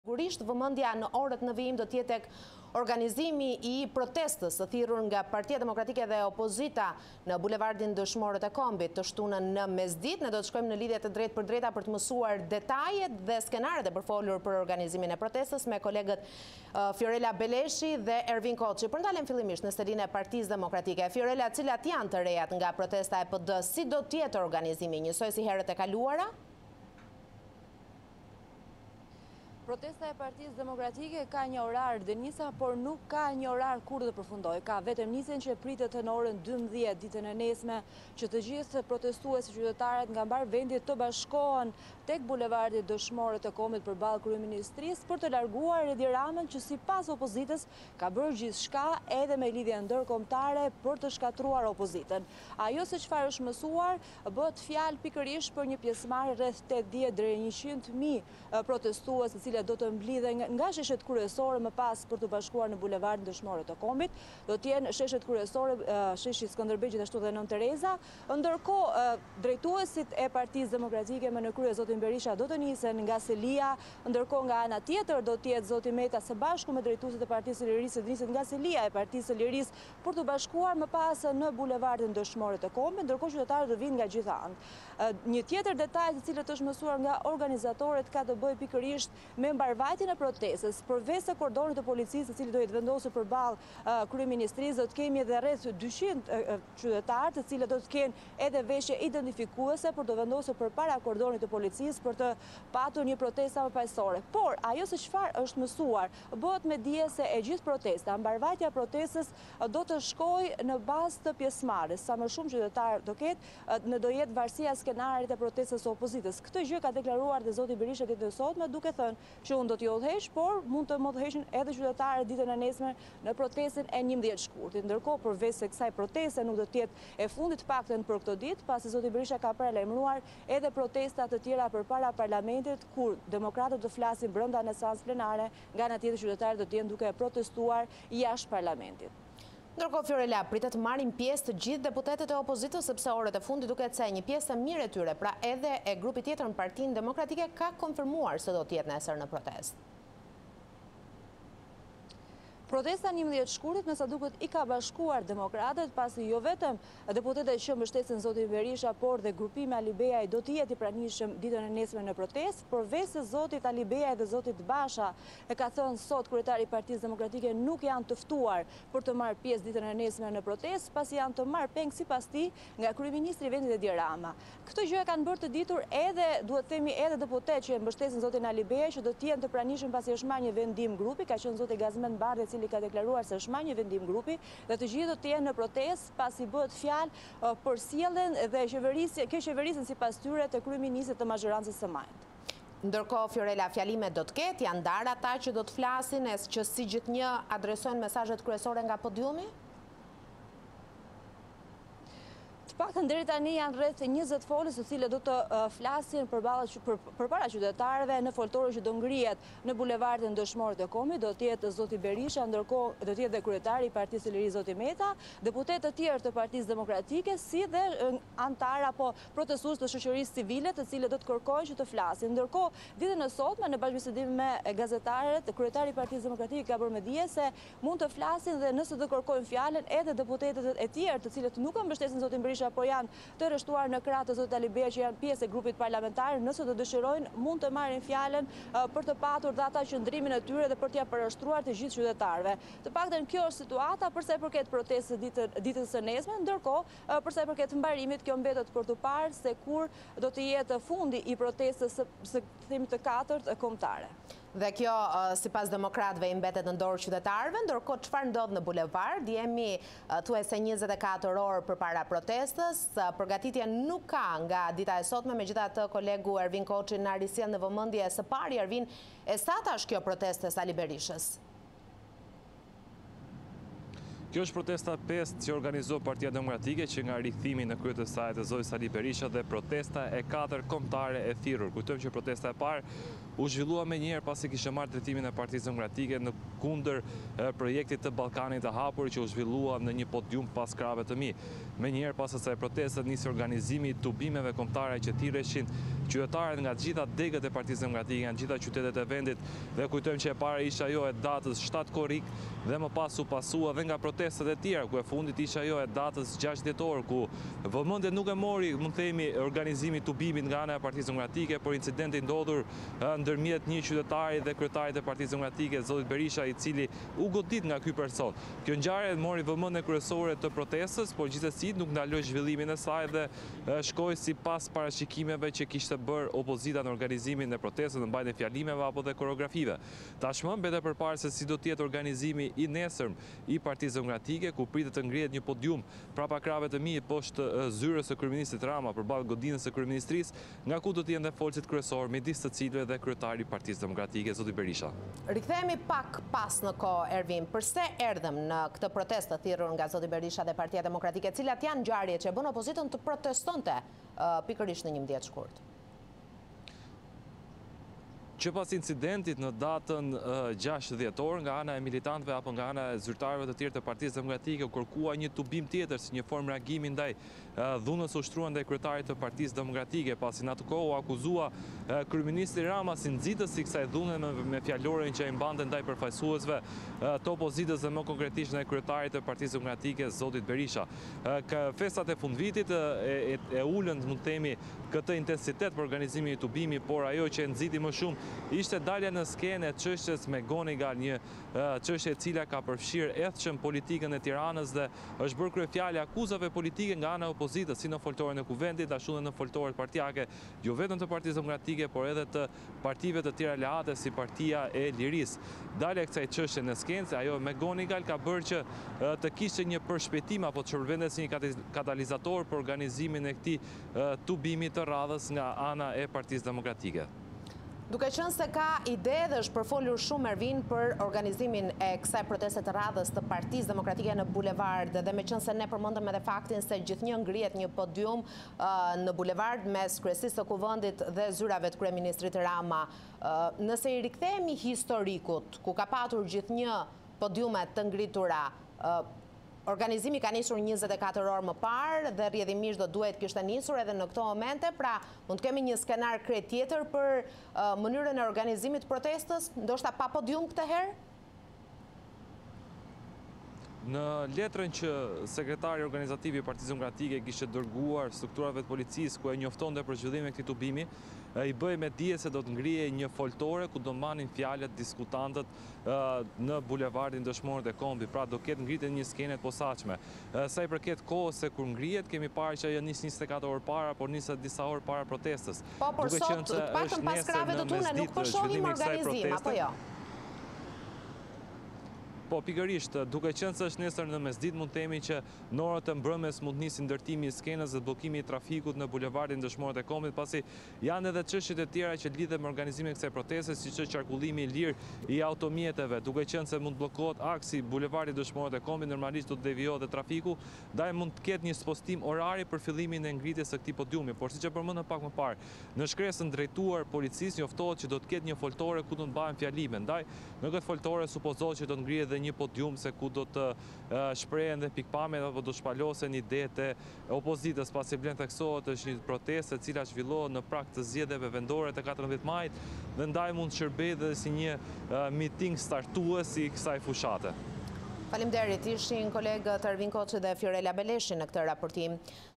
Opozita në protestë, Berisha thërret qytetarët kundër qeverisë së Edi Ramës
Të shtunën në mesditë, Partia Demokratike dhe Sali Berisha kanë thirrur qytetarët në protestë.

Çështja McGonigal është pika që mbushi kupën, sipas demokratëve që kërkojnë dorëheqjen e kryeministrit Edi Rama. Fjorela Beleshi dhe Ervin Koçi raportojnë detajet rreth përgatitjeve.